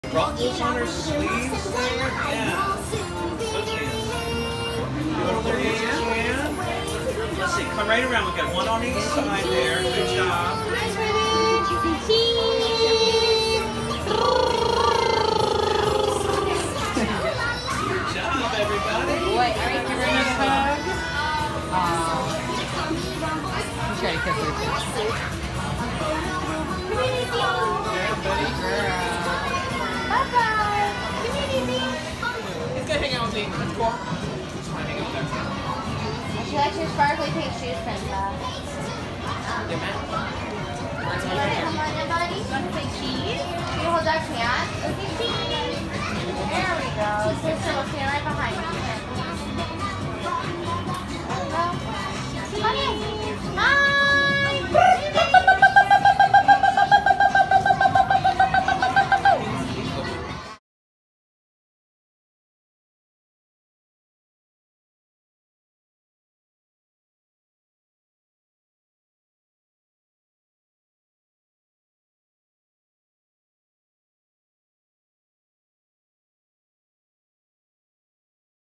Meet and Greet with Doc Mcstuffins, Fancy Nancy and Vampirina at Hollywood Studios in Disney World
The kids got to meet a few of their favorites in Hollywood Studios in December 2019. We can't wait to have meet and greets again.